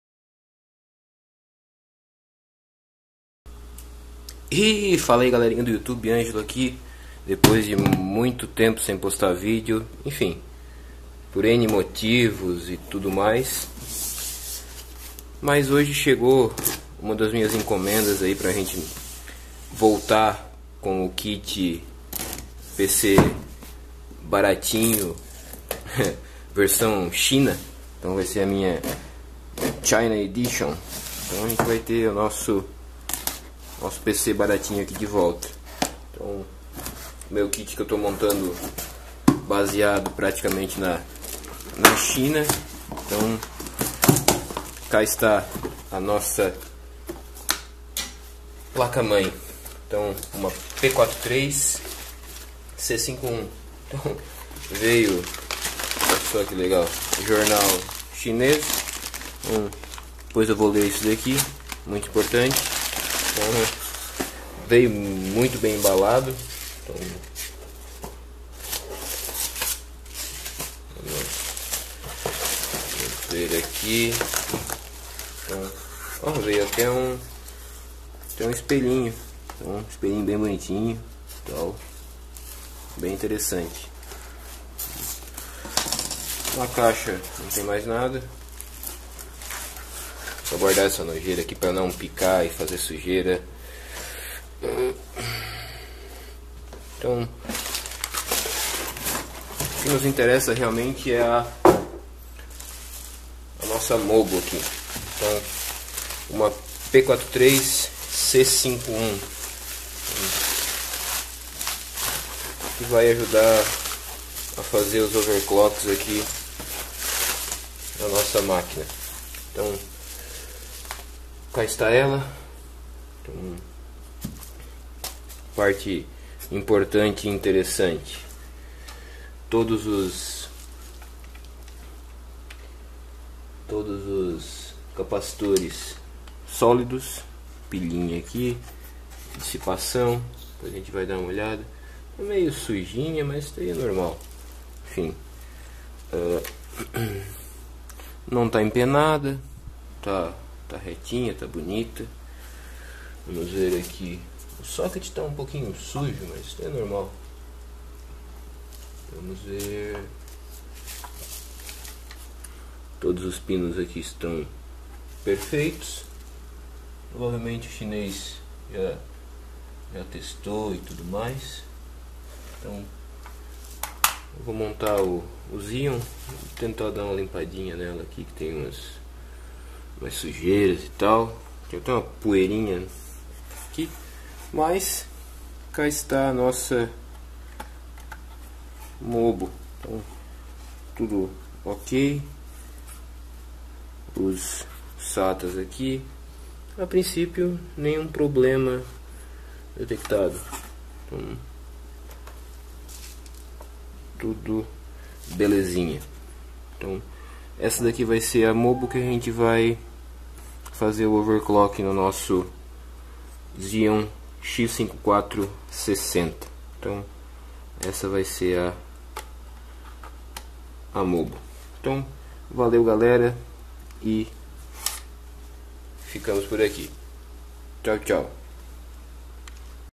Unboxing MSI P43-C51 - Placa mãe da China - Aliexpress
Placa mãe que comprei na China, paguei 160 reais mais 40 reais de impostos.